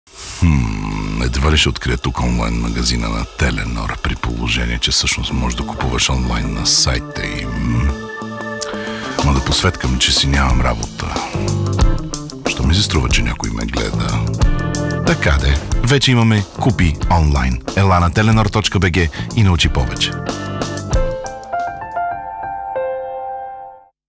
3, 2, 1 - старт! Купи онлайн
Купи онлайн на telenor.bg. Сега можеш да поръчаш лесно и удобно нови смарт устройства от нашия сайт, а ние ще ти ги доставим до вратата. Освен това, вече можеш да подновяваш и договора си онлайн. Не чакай, влез